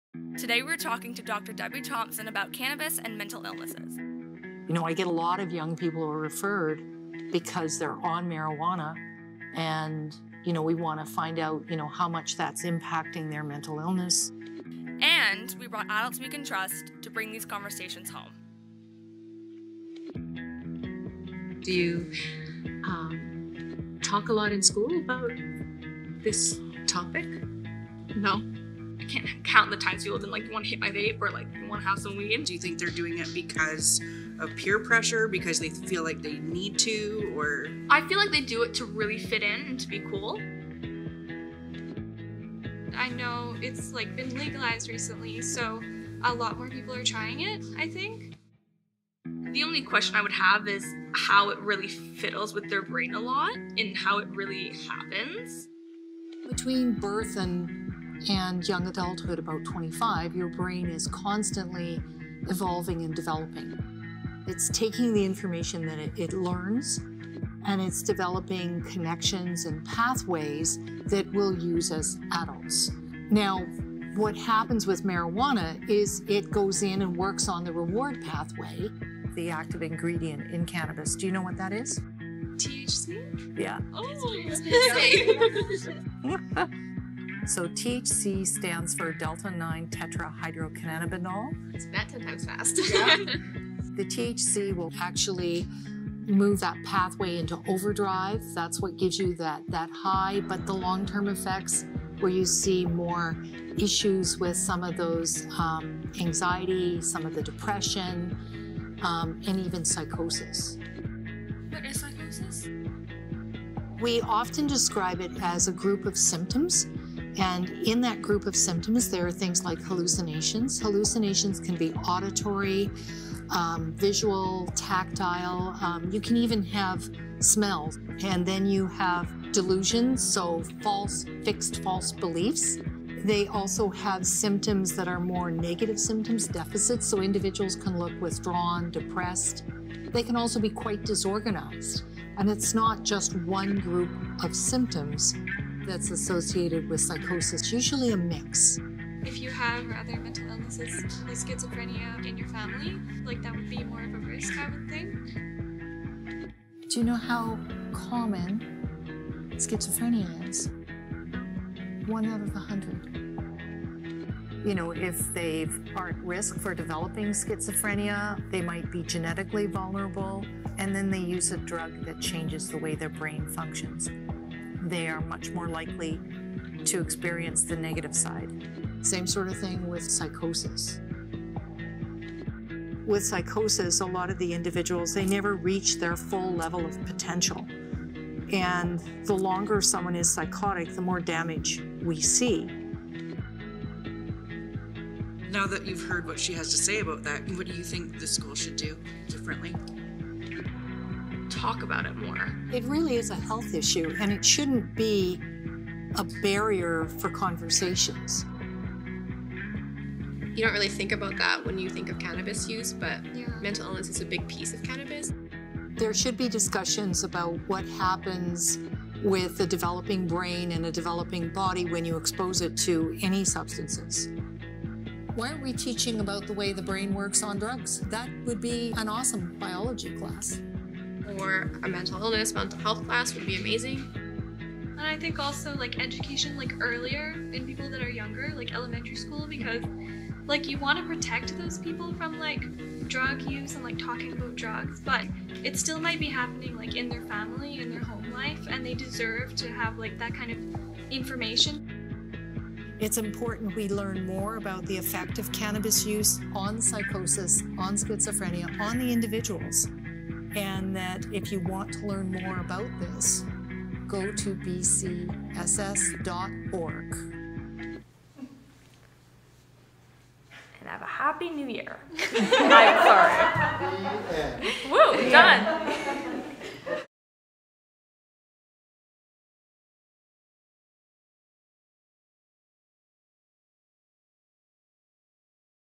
BC Schizophrenia Society -- Cannabis and Mental Illness
Is cannabis use safe for everyone? Can it impact our brains? What about mental illness? Cannabis use can directly impact those who are predisposed to mental illness, and it is important to understand what our risks are before use. Learn about cannabis use and mental illness as we discuss the topic with Dr. Deborah Thompson in a conversation led by our youth.